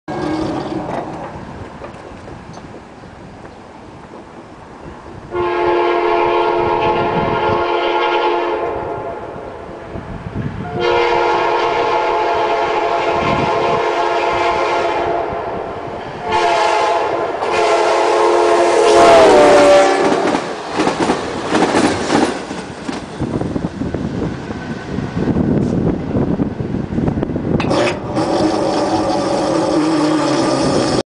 Amtrak 90418 does 79!
An Amtrak Cabbage leads three coaches and am AMD103 in Ogden Dunes